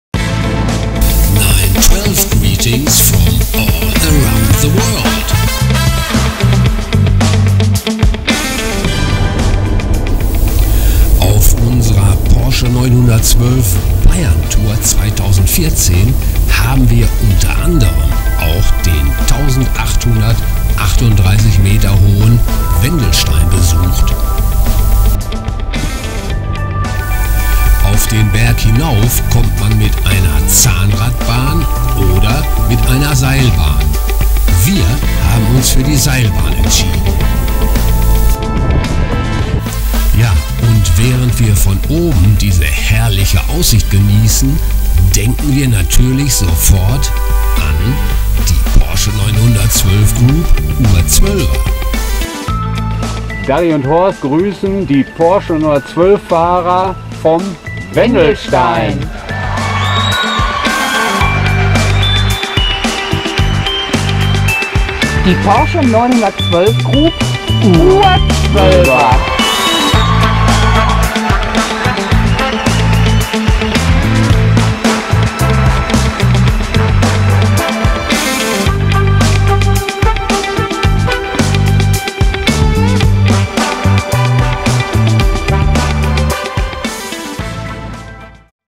912 Porsche Greetings from Wendelstein
HORST-TV presents an other 912-Porsche-Greetings-Video
from Wendestein-Bavaria.
Horst & Daggi unterwegs "912-Bavario-Austria-Tour 2014"